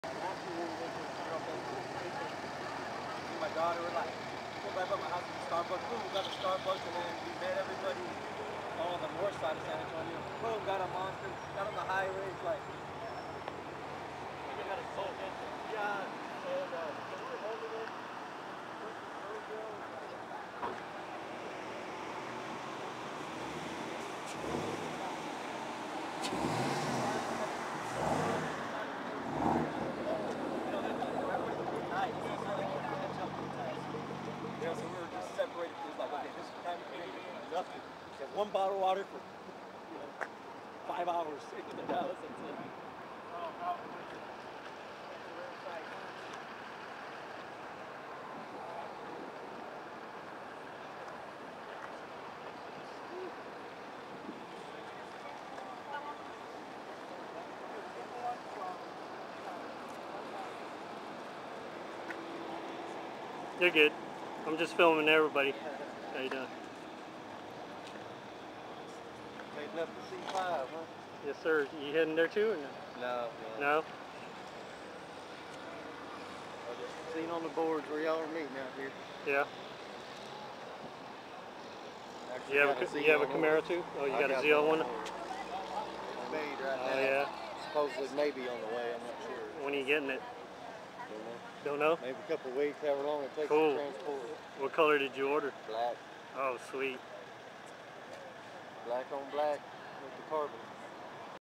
Camaro5 Road Trip - Texarakana, TX
A quick pit-stop by the Texas 5th Gen Camaro Clubs from San Antonio, Austin, Houston, and Dallas, on the way to Indianapolis, IN for the 3rd Annual Camaro5Fest.